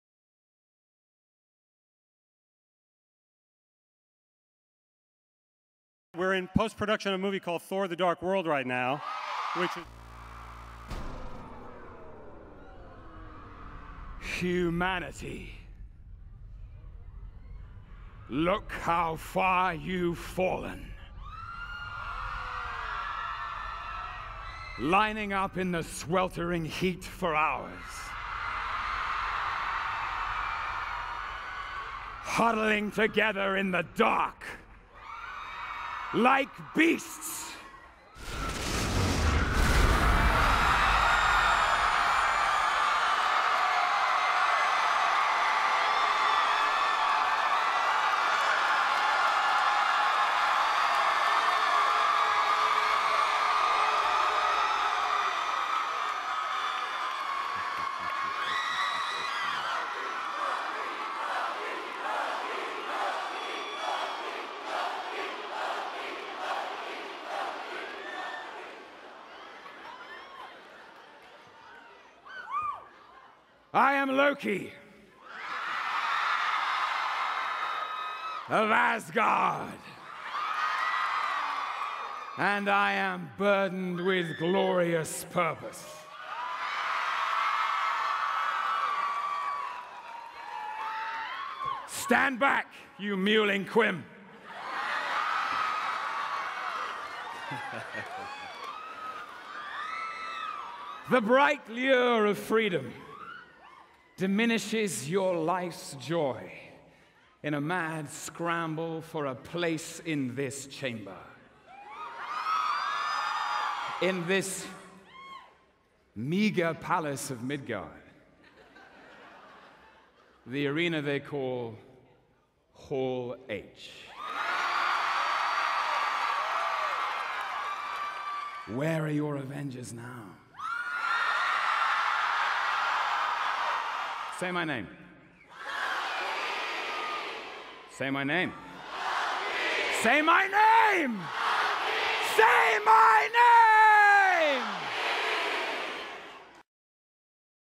Локи на фестивала на комиксите Comic-Con 2013
Вижте как Локи изненада многобройните фенове в най-голямата зала "H" по време на фестивала Comic-Con 2013 :) Премиерата на "Тор: Светът на мрака" в България е на 1 ноември, в кината на 3D.